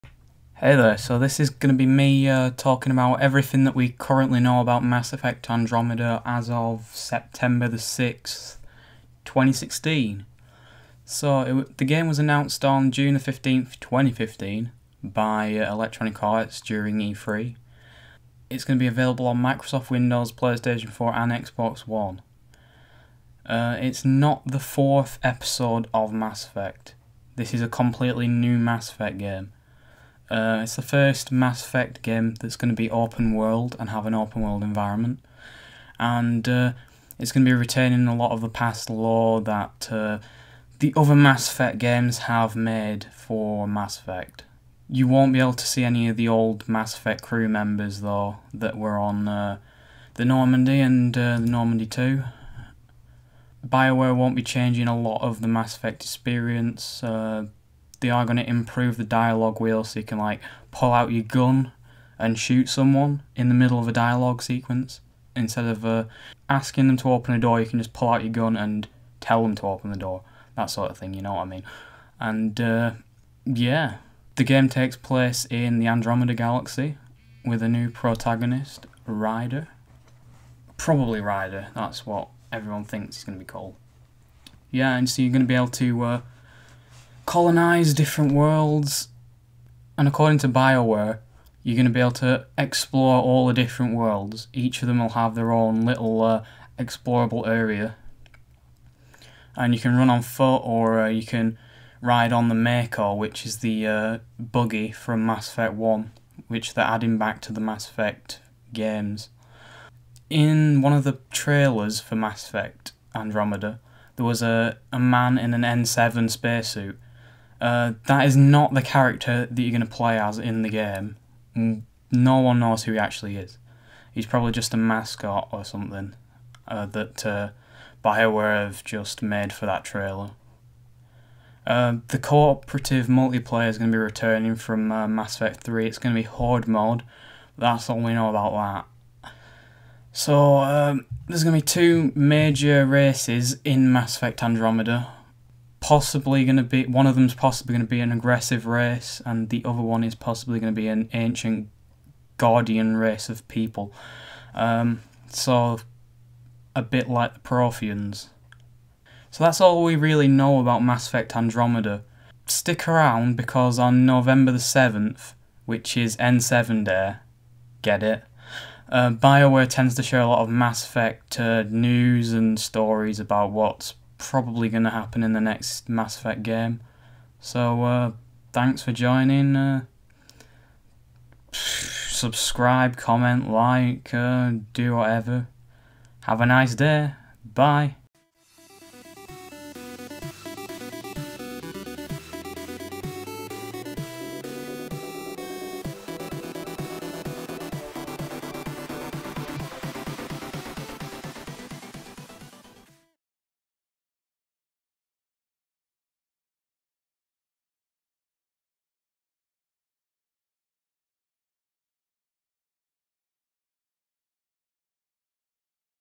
What We Know About Mass Effect Andromida!!!
everything we know about mass effect Andromeda as of September 6 2016